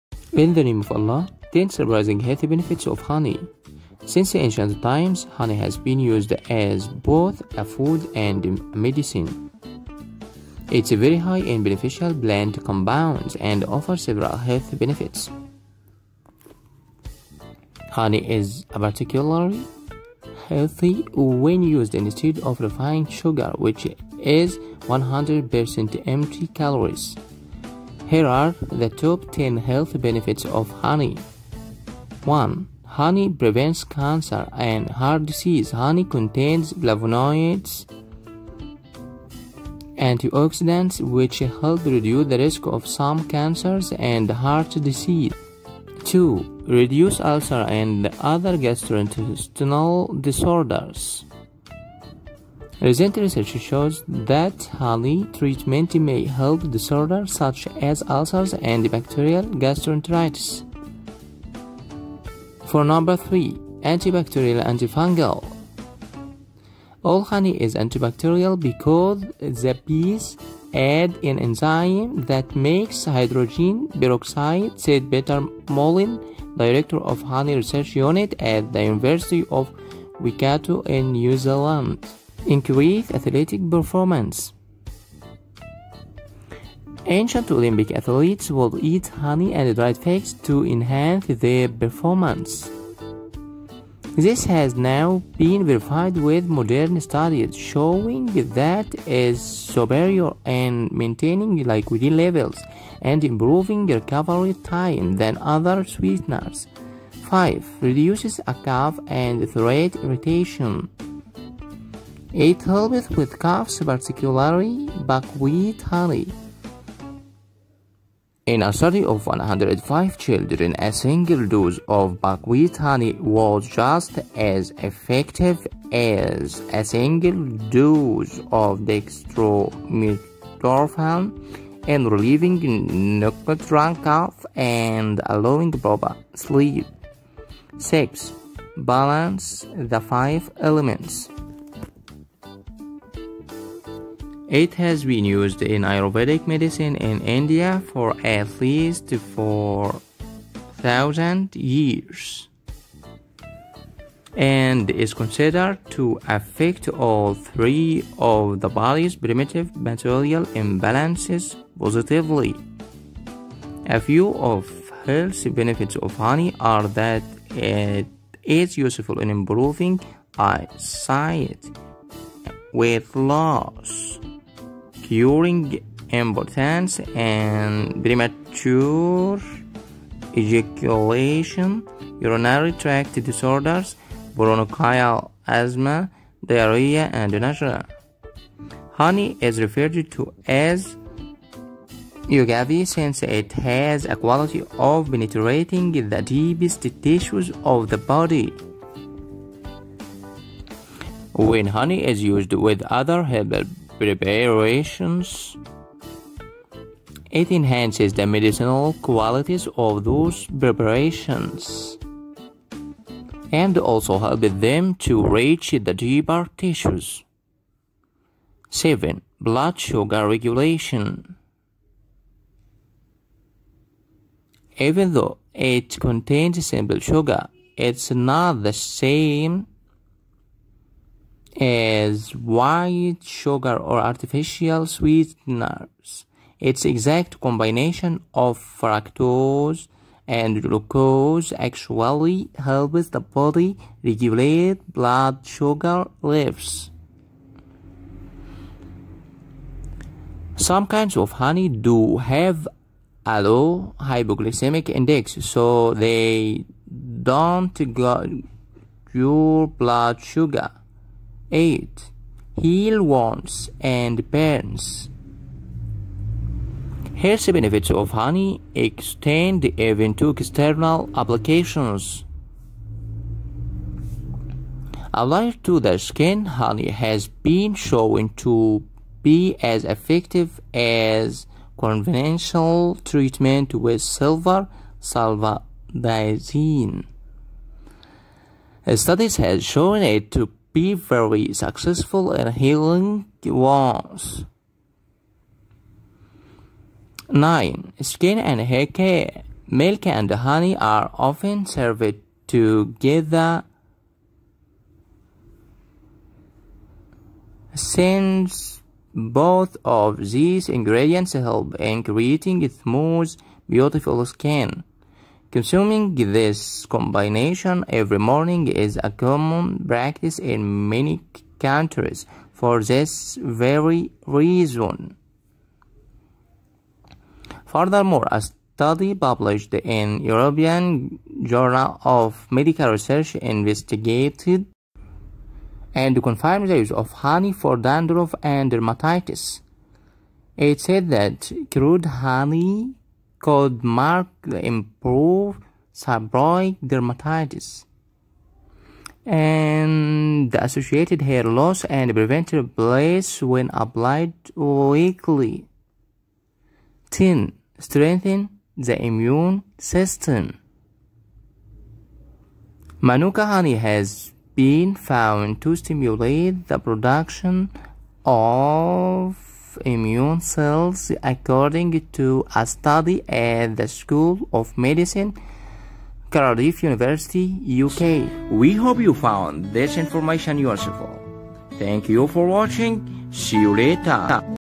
See what will happen to you from benefits when eating honey daily
د.احمد عطيه الدهشان
هو مؤسس ورئيس الجمعية الطبيه العموميه للتغذية العلاجية وباحث في العلوم الطبيه الحيوية في اسطنبول